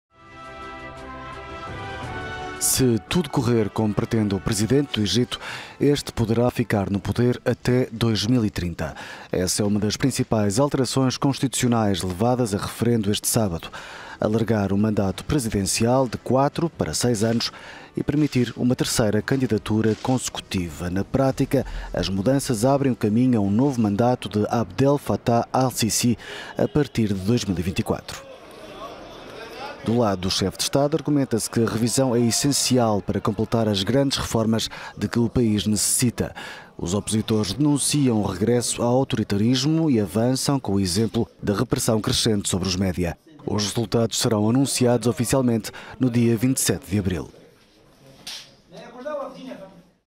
Referendo no Egito pode dar terceiro mandato a al-Sisi
Egípcios votam num referendo que pode dar ainda mais poderes ao presidente. Opositores denunciam o regresso ao autoritarismo.…